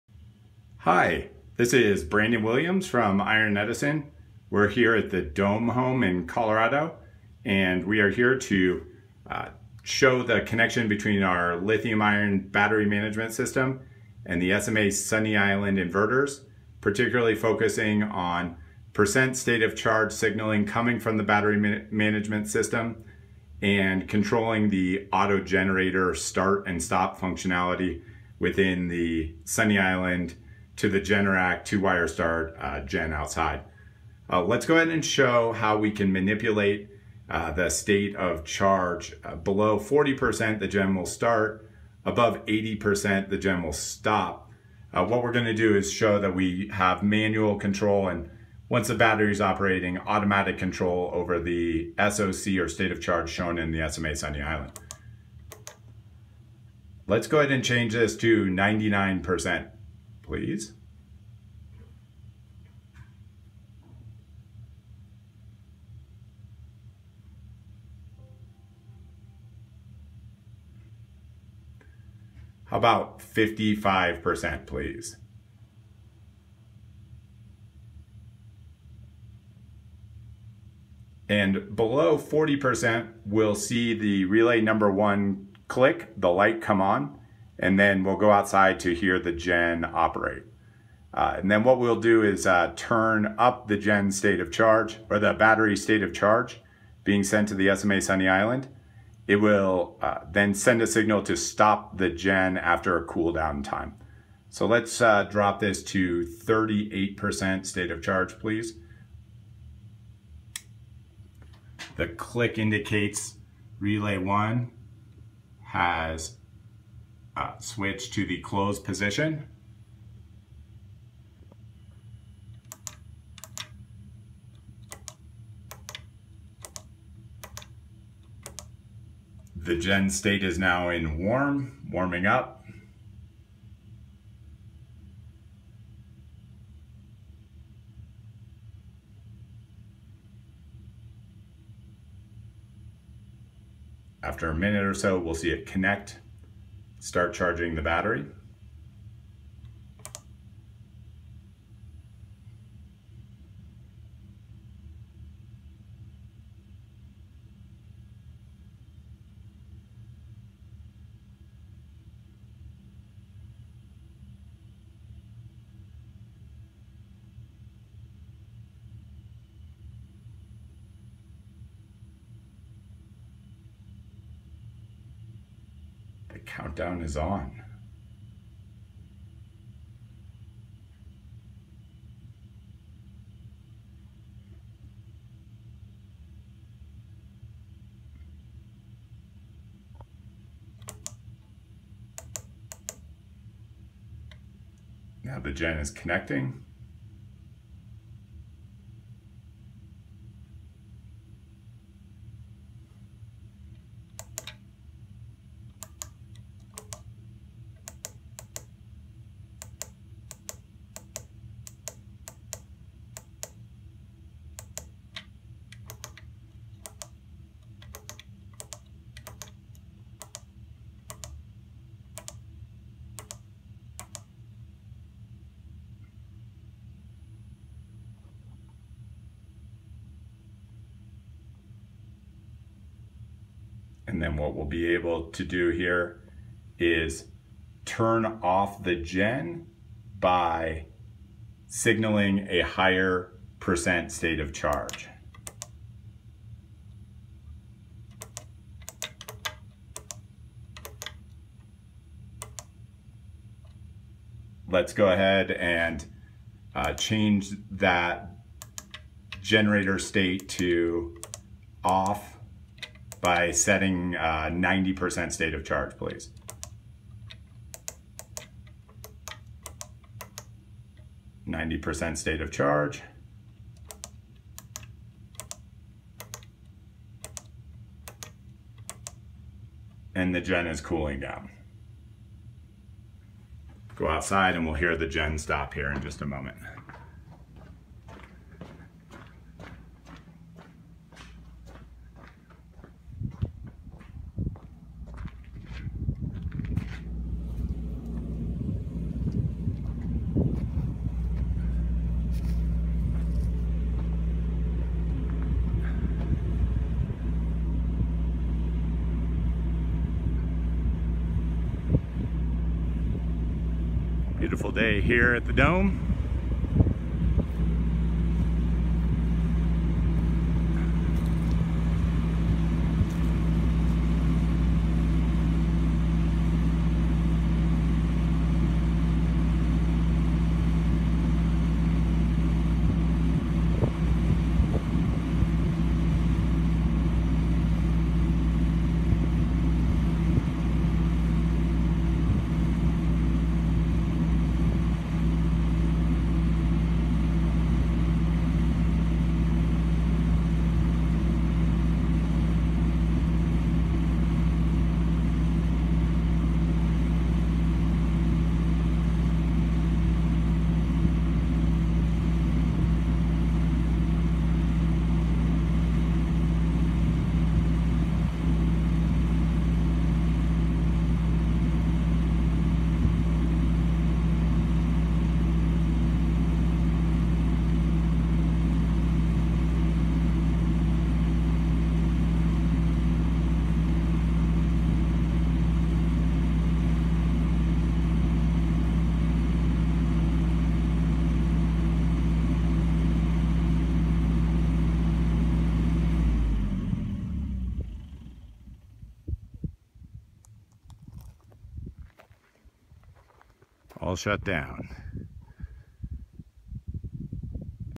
Solar Battery - SMA Sunny Island to Lithium BMS to Generator AutoStart
In this video we show how the Iron Edison battery management system sends State of Charge messages via CANBUS protocol to the SMA Sunny Island battery inverter / charger.  The Sunny Island relies on state of charge (SOC) data to make all it's internal decisions on wether to charge / not charge, sell / not sell, call for generator / stop gen, just to name a few.  It's a significant advancement for lithium battery systems to have a more reliable state of charge data shared between the battery and Sunny Island.  More info at IronEdison.com